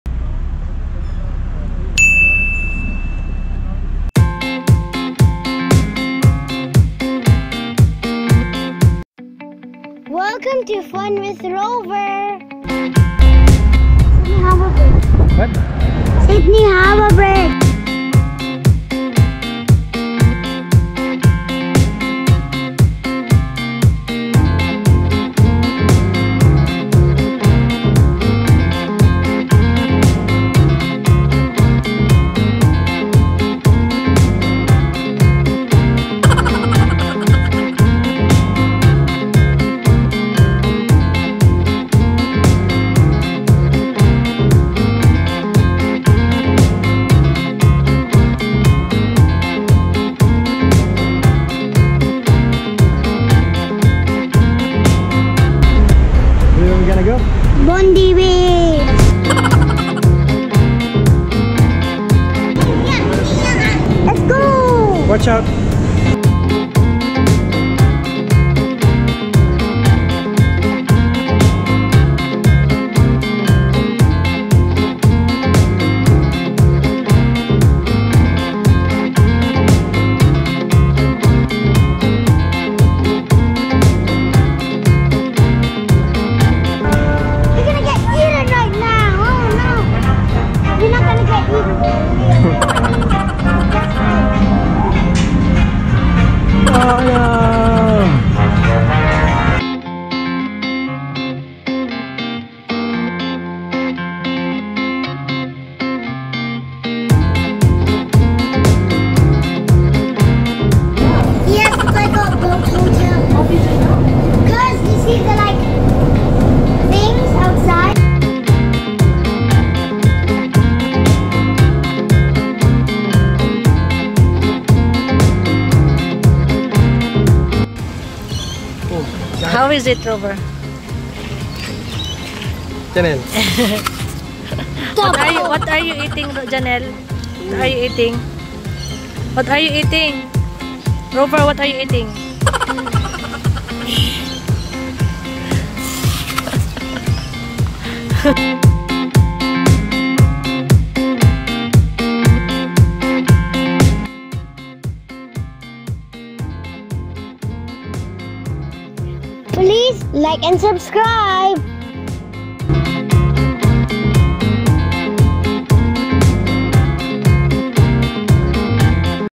Fun with RoVer: Jamming On Big Bus Sydney, exploring Sydney with The Floss and The Hype Dance Steps
Rover on Big Bus Sydney, QVB, St Mary Cathedral, Sydney Eye Tower, Luna Park, Opera House, The Rock, Ferry Tour and more. While doing the Floss and The Hype Dance.